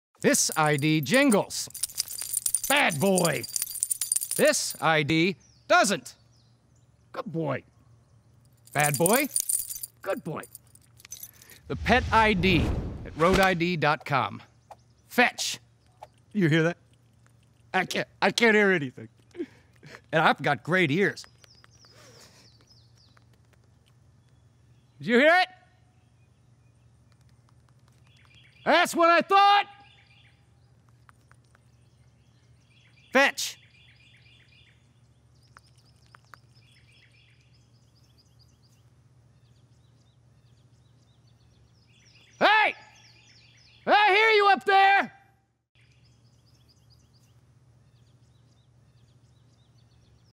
The Silent Pet Tag Solution from ROADiD.com
Your pup’s cheap, dangly dog tags are an insult to ears everywhere. Meet the Pet ID: custom dog tags for the modern world.

Preserve your sanity with jingle-free pet ID tags that are whisper-quiet, good-looking, and backed by a LIFETIME GUARANTEE. Thread your pooch’s collar through two elastic loops for a sleek, low-profile dog ID tag that won’t get snagged during daily canine shenanigans. Every engraved dog tag is available in three faceplate finishes—Slate, Graphite, and Rose Gold—and comes in two sizes to accommodate every breed and body type. Lose the jingle with the Pet ID.